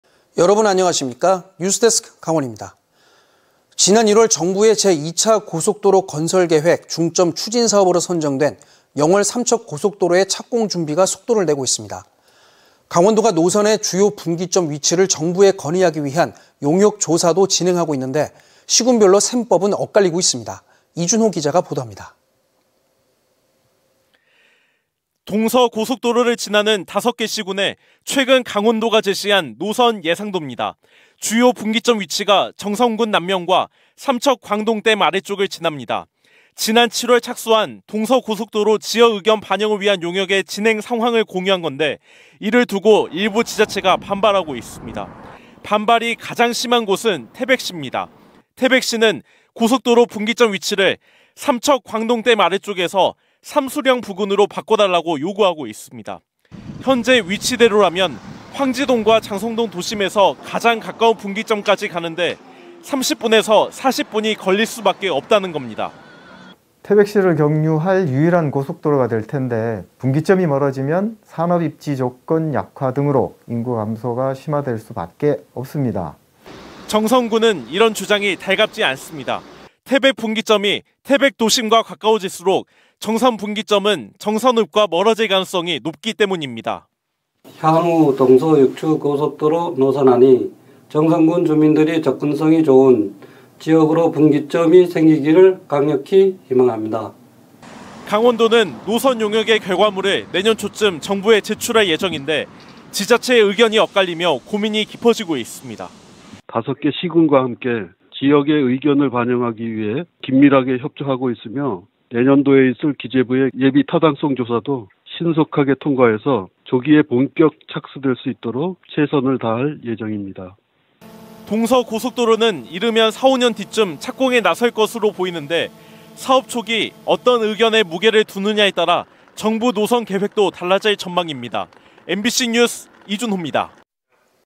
[리포트]동서고속도로 노선 지자체 간 셈범 엇갈려 20221120
지난 1월 정부의 제2차 고속도로 건설계획
중점 추진사업으로 선정된 영월~삼척 고속도로
착공 준비가 속도를 내고 있습니다.

강원도가 노선의 주요 분기점 위치를
정부에 건의하기 위한 용역 조사도
진행하고 있는데 시군별 셈법은 엇갈리고
있습니다.